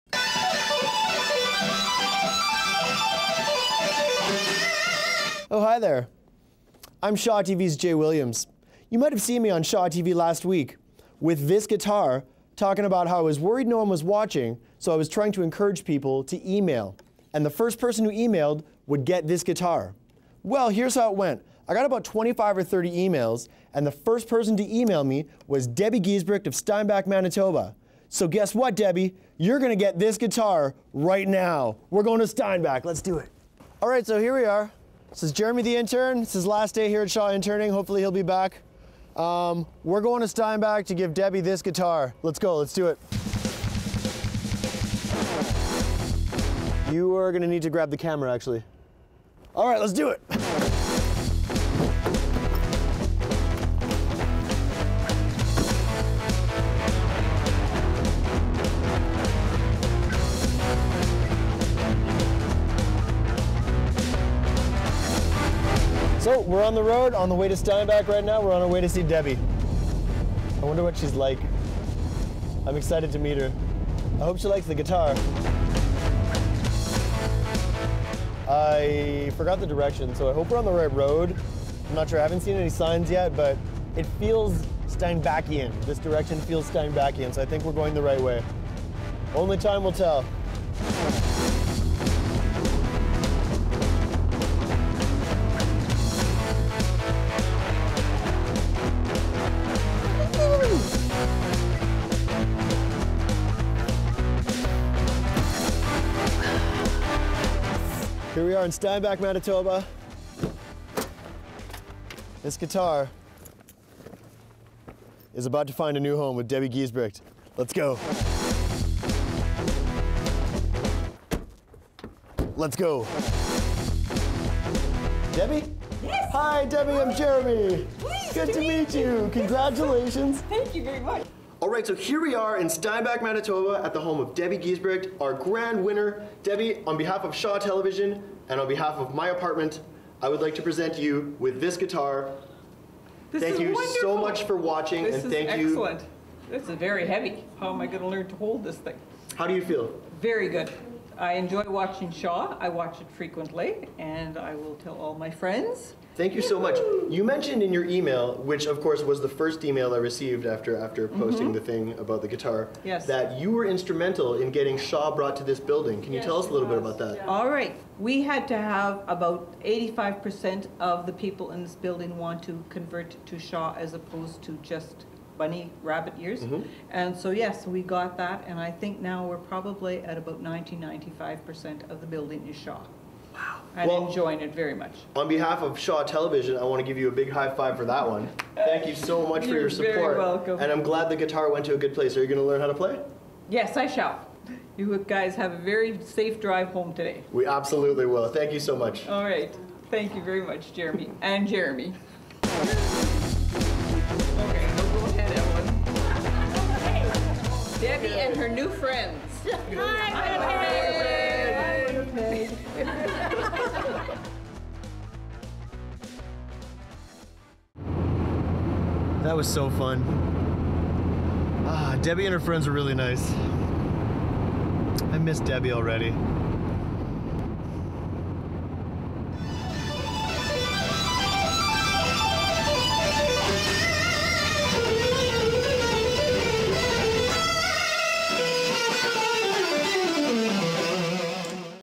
Guitar Drop Off
J.Williams travels to Steinbach MB to deliver his guitar to Grand Prize Winner Debbi Giesbrecht